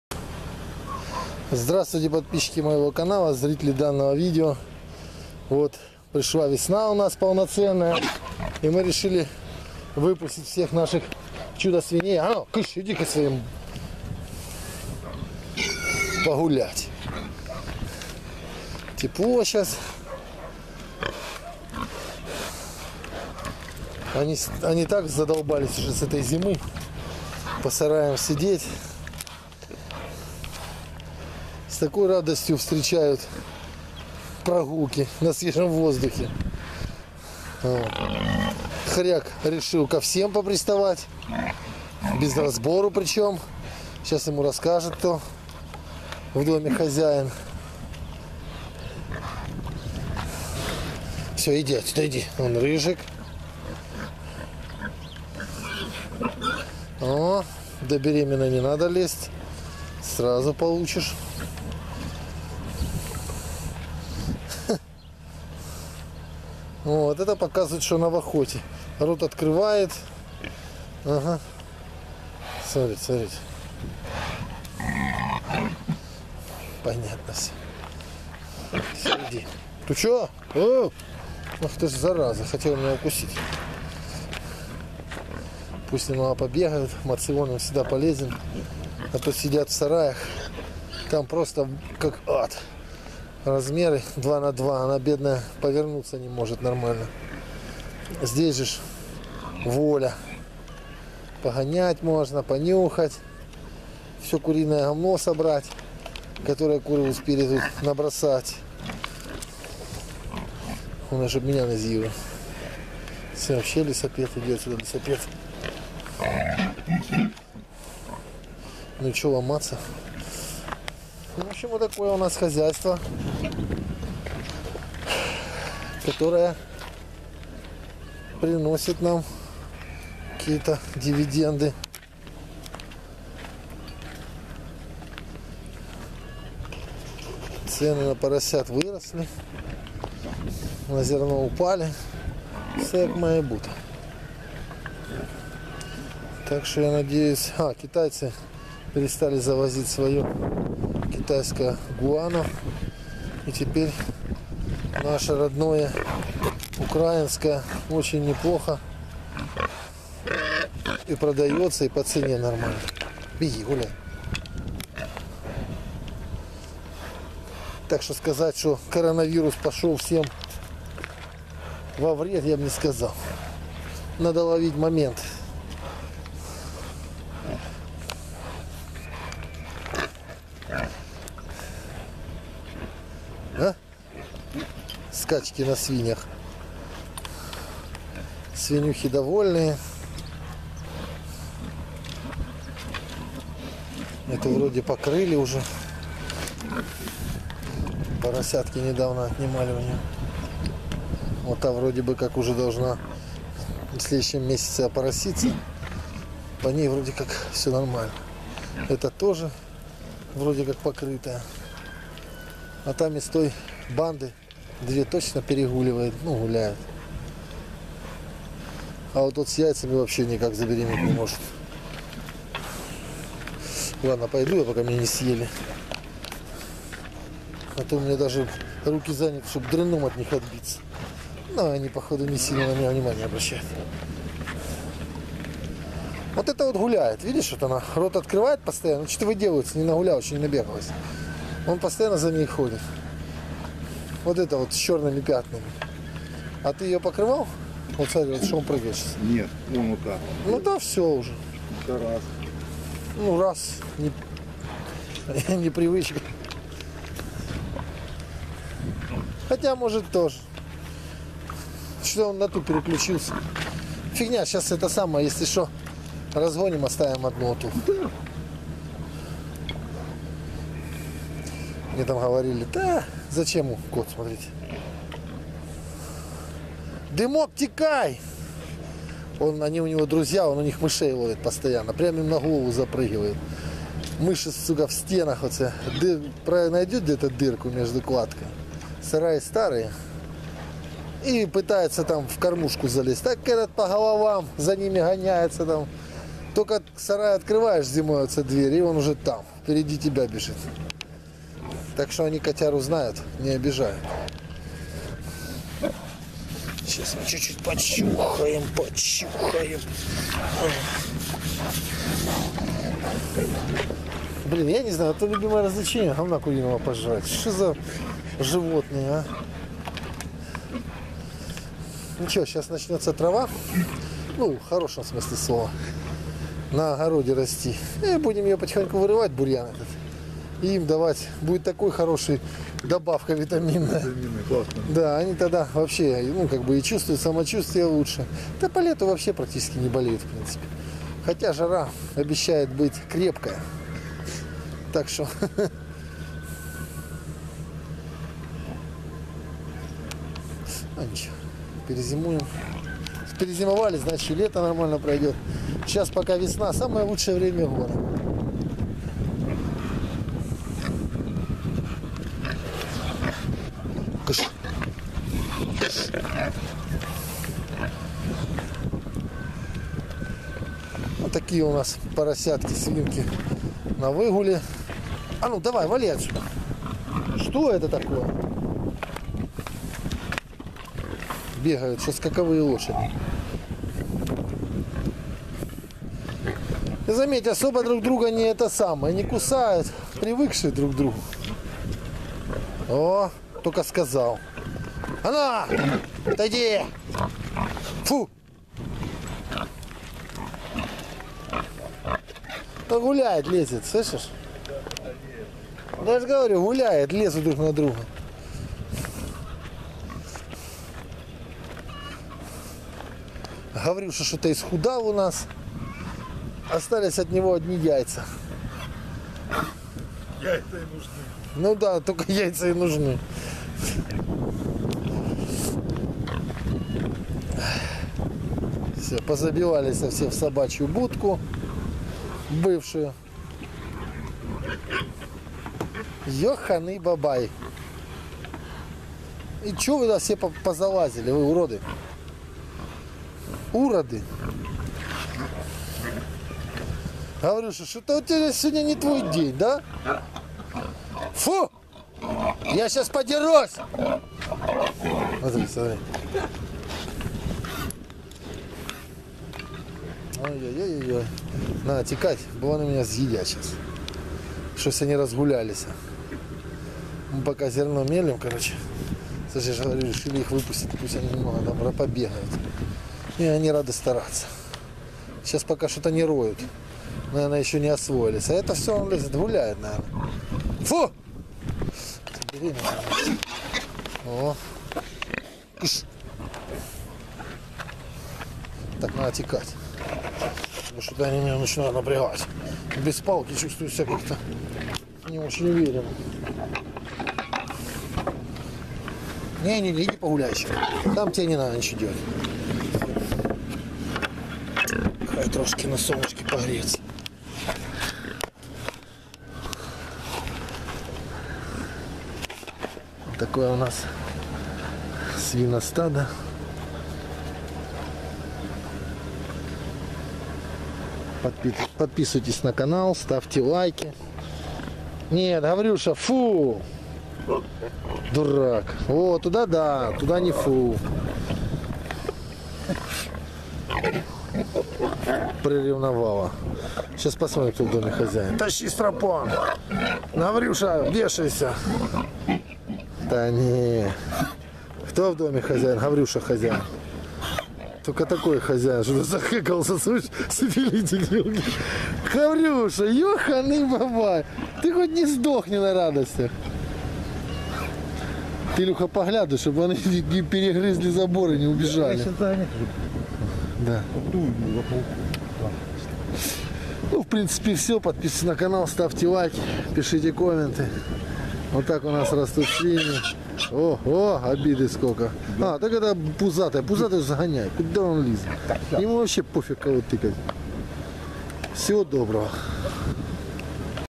Свиньи на свободном выгуле после длинной зимы в первые дни весны.
Наконец то наступила весна и мы решили сделать праздничный выгул для свиноматок .Радости не было границ и они чуть не сожрали меня как обычно.Но всё закончилось хорошо ,свинки набегались напрыгались и радостные пошли завтракать.

Finally, spring came and we decided to make a festive walk for sows. There were no boundaries of joy and they almost ate me as usual. But everything ended well, pigs ran in and jumped and joyful went for breakfast.